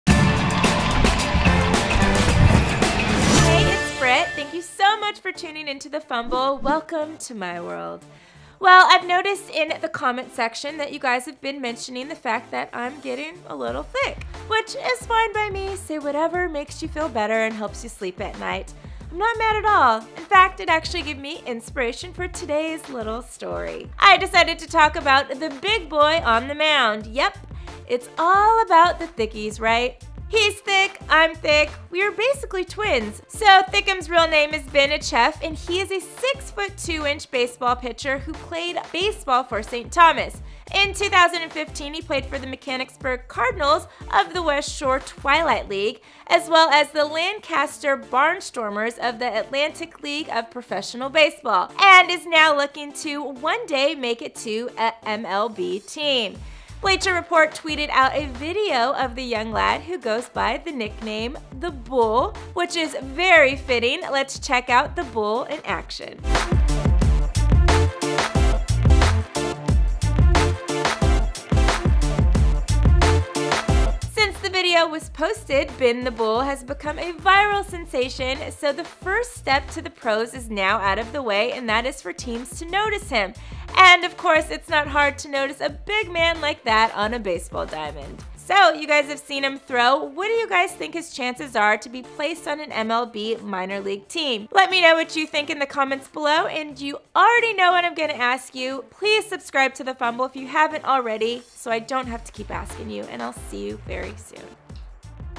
300-Pound Pitcher Ben 'The Bull' Ancheff Sets His Sights on MLB
Ben Ancheff, nicknamed 'The Bull,' is a 6'2" baseball pitcher who has bounced around amateur and minor leagues, but thanks to some online recognition, he's hoping to catch the eye of major league scouts. Given his size, does he have a legitimate shot? Does his size matter that much for his position? Let us know what you think his chances are in the comments section below.